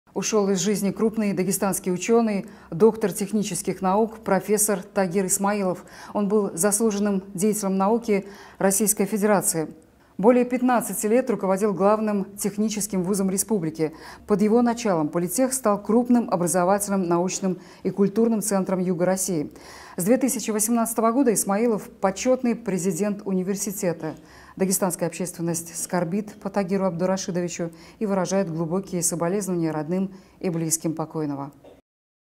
Умер бывший ректор ДГТУ Тагир Исмаилов
Ушел из жизни крупный Дагестанский ученый, доктор технических наук, профессор Тагир Исмаилов. Он был заслуженным деятелем науки РФ. Более 15 лет руководил главным техническим вузом республики. Под его началом политех стал крупным образовательным, научным и культурным центром юга России. С 2018 года Исмаилов - почетный президент университета.

Дагестанская общественность скорбит по Тагиру Абдурашидовичу и выражает глубокие соболезнования родным и близким покойного.